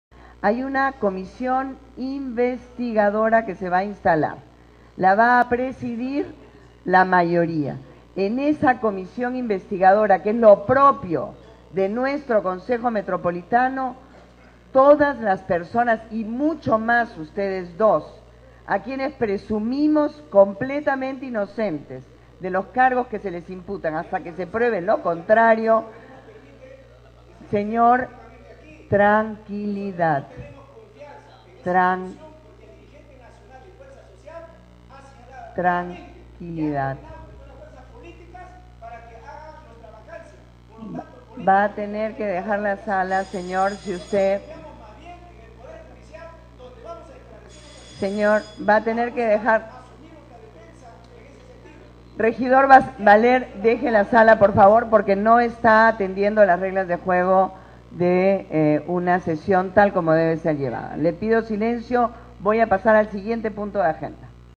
Regidor Valer se enfrenta a la alcaldesa Susana Villarán
La alcaldesa reiteradamente le pidió calma al regidor Valer sin embargo este hacía caso omiso y seguía con su reclamos en alta voz. Se le tuvo que pedir que se retire de la sala, sin embargo se sentó en su curul guardo silencio y la sesión pudo continuar como si nada hubiere pasado.